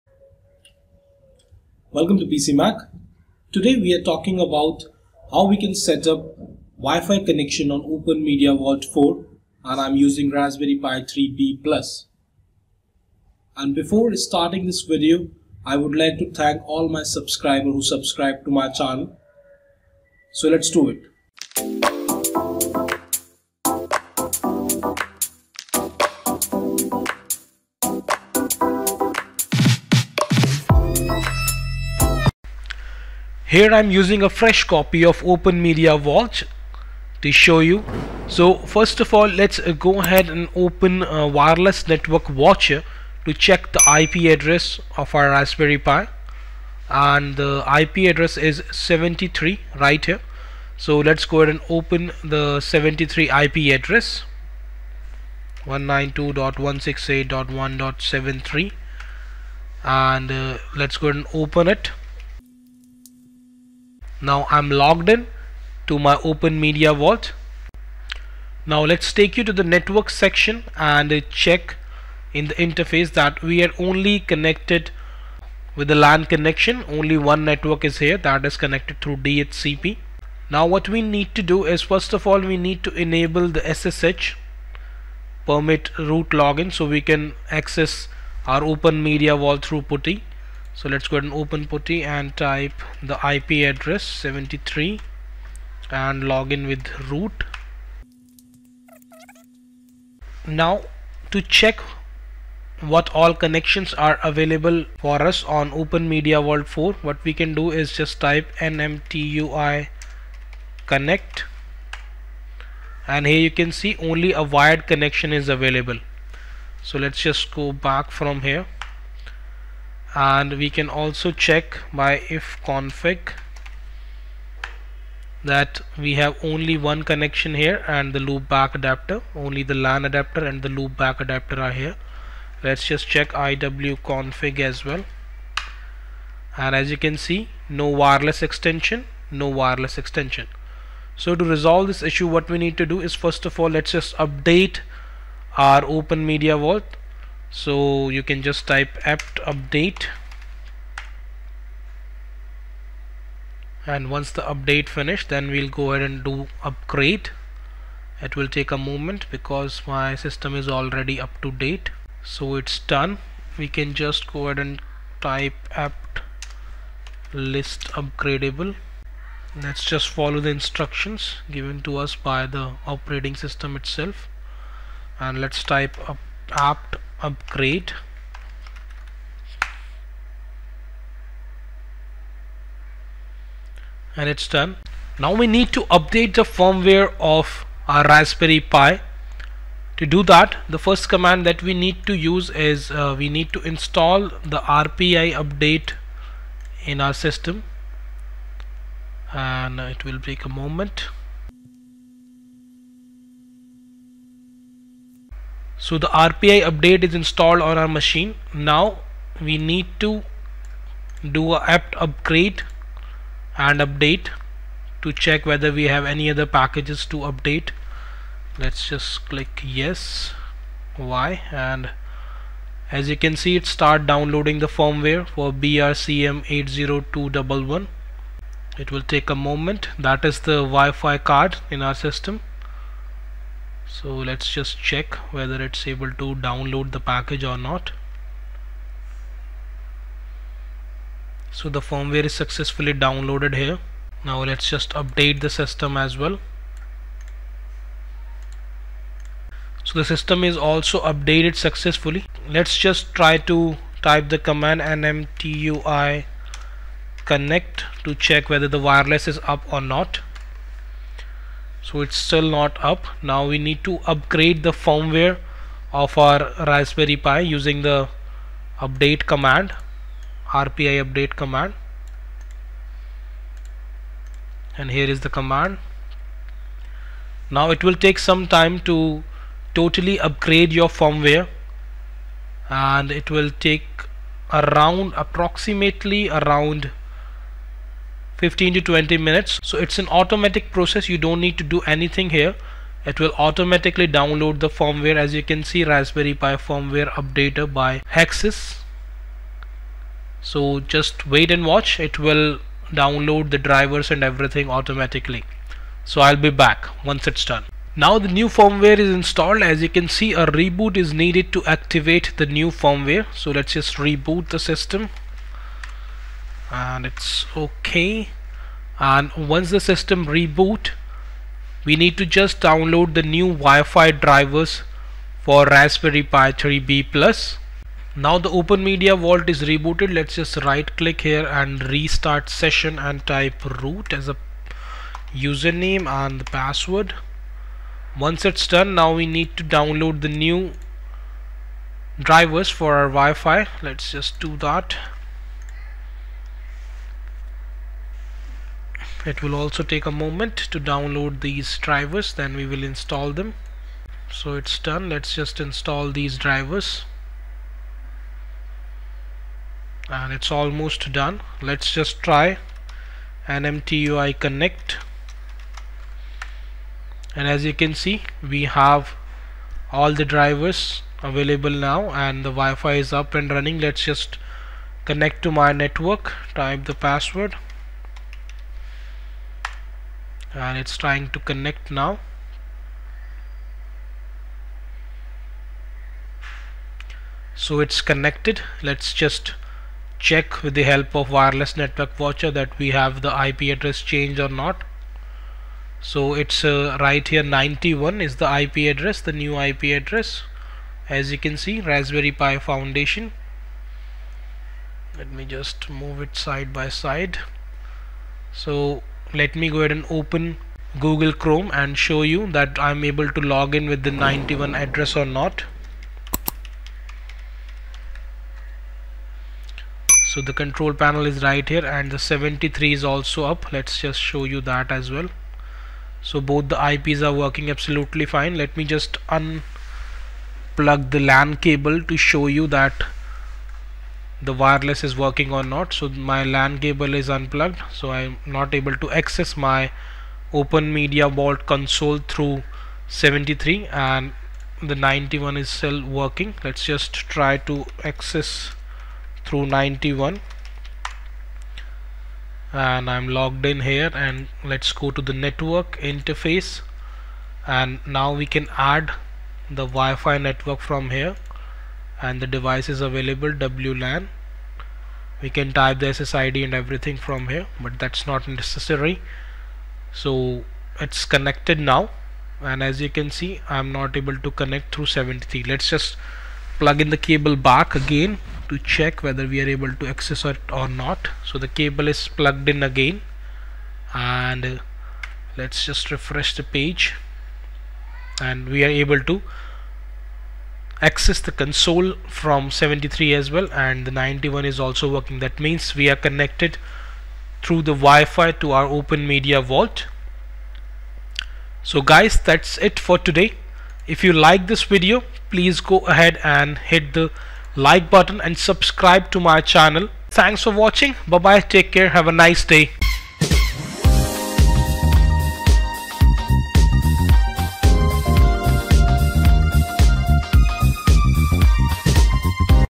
Openmediavault WiFi Setup | Raspberry Pi 3B+
In this video we are going to setup WiFi on Openmediavault 4.

Follow Us On!